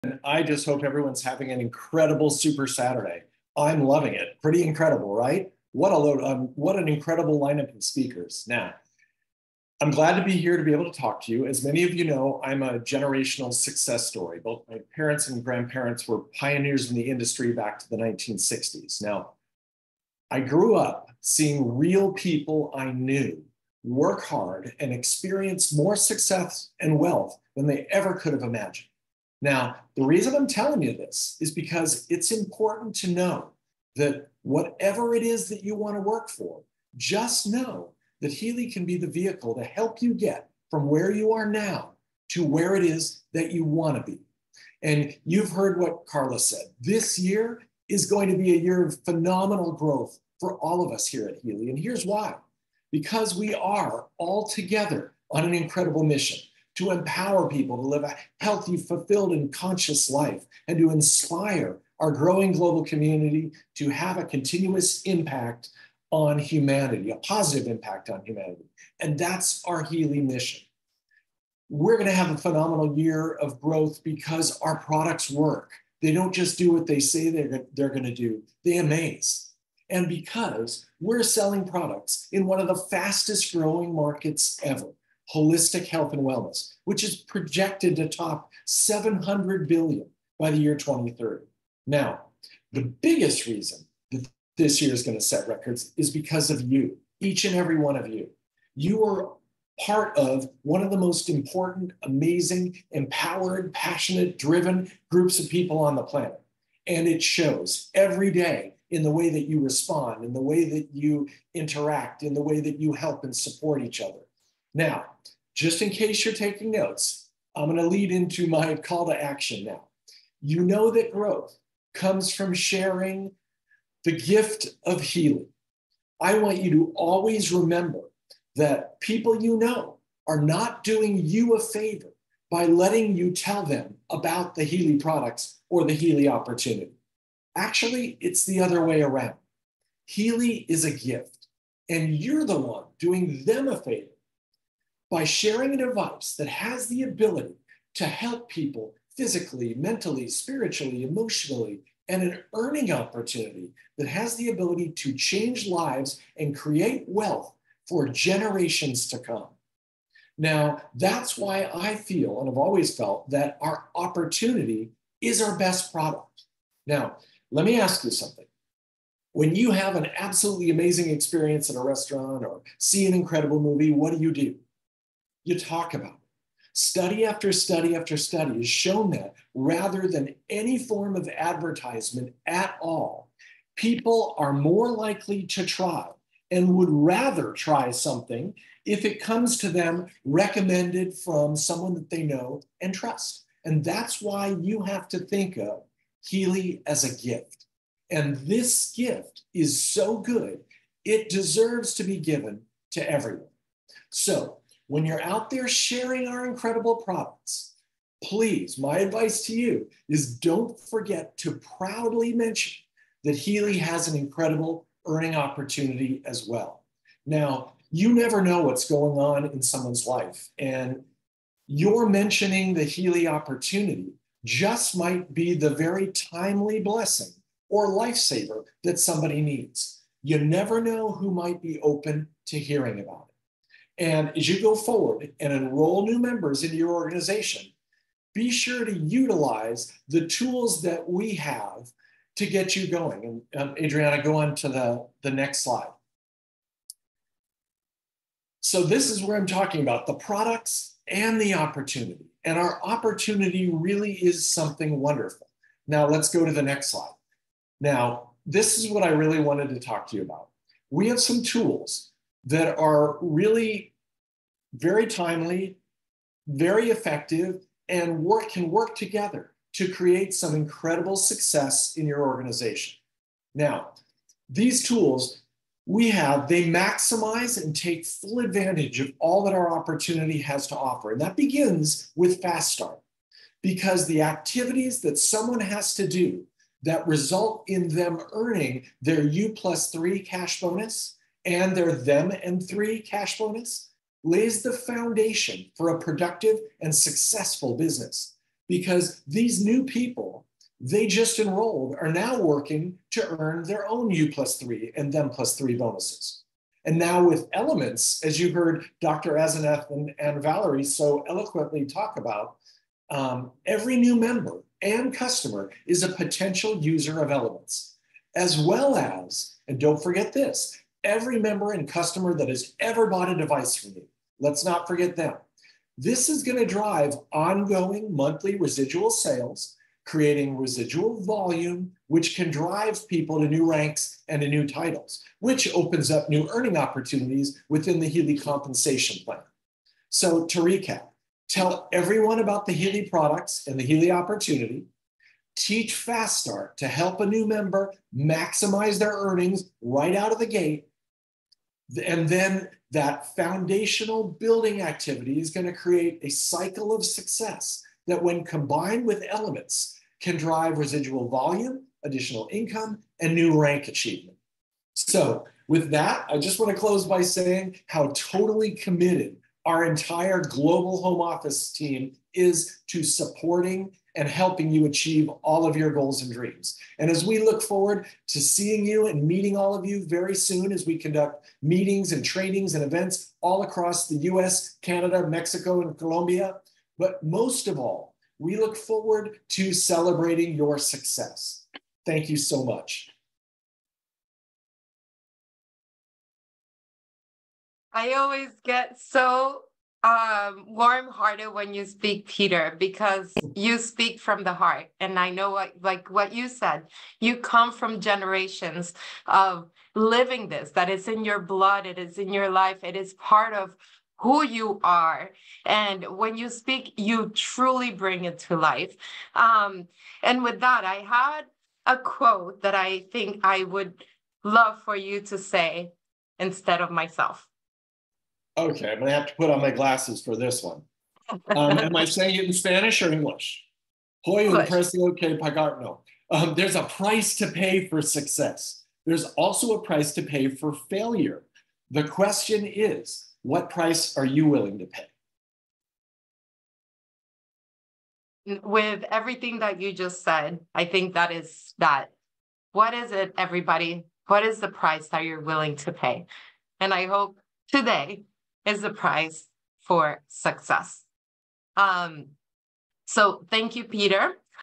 Healy Super Saturday - The Power of the Healy Opportunity with Peter Christoff, Managing Director NA
Peter Christoff, our Managing Director for North America, speaks to Healy World Members at our Super Saturday event on June 22nd, 2024!